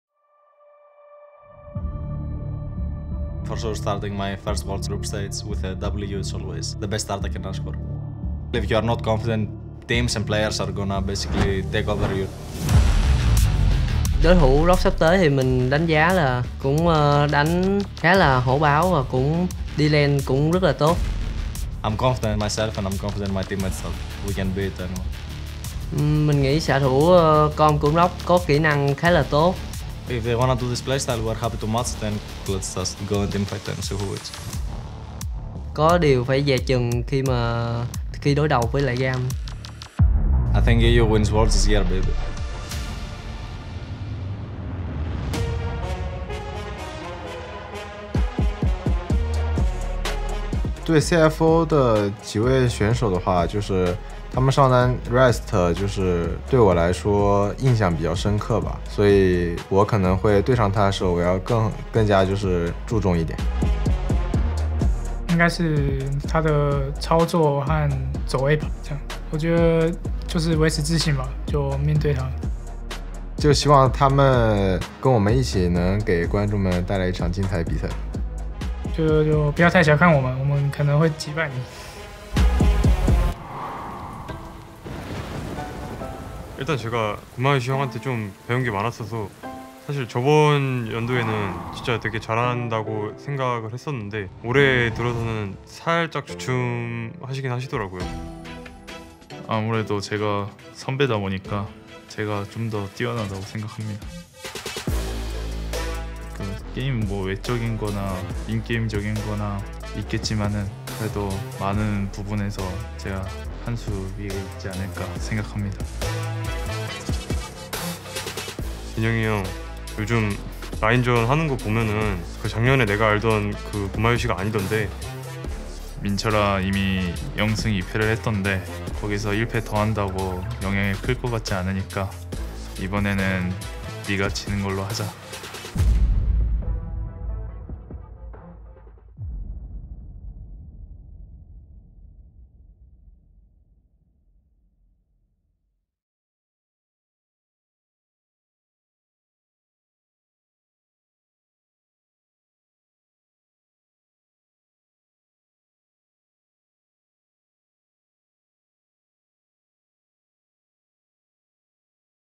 Rogue 正在小組賽裡橫行霸道 | Worlds 2022 小組賽 Day 3 前導預告
有什麼比兩個賽區第一種子之間的對決更讓人興奮的呢？今天 VCS 和 LEC 的冠軍隊伍將在召喚峽谷上對峙。打法殘酷卻具備紀律性的隊伍將對上肆無忌憚且充滿侵略性的隊伍，但哪種打法風格是更加具備優勢的呢？只有一支隊伍能夠贏下勝利，誰將會展現出生存的本能？

每支隊伍總會有對上強豪隊伍的一天。CTBC Flying Oysters 將在今天面對如此的敵人。RNG 是 MSI 的衛冕冠軍，而他們依然持續保持住這個頭銜。CFO 是本屆初次參戰世界大賽的隊伍，無人能曉得他們面前的命運將會是如何。其中一支隊伍已經立下了他們的期許，而另一支隊伍的目標則是定下他們的期望。全世界都在關注 CFO 的表現，他們能否成功辦到呢？

在今天的比賽中，將會進行 A 組最重要的賽事之一。LCS 的代表隊伍 Cloud9 將要對上 LCK 的傳奇 T1。他們今天的對決將會是一場攸關排名以及榮耀的比賽。Cloud9 的 AD 選手 Berserker 將要面對他的前隊友 T1，以及他的大學長 Gumayusi。雙方都已經熱血沸騰了，以前的學弟是否要超越學長成為大師了呢？

小組賽：10 月 7 日至 10 日以及 10 月 13 日至 16 日
八強賽：10 月 20 日至 23 日
四強賽：10 月 29 日與 30 日
冠軍賽：11 月 5 日

記得追蹤 LoL Esports 的各大社群帳號來獲得最新的世界大賽消息：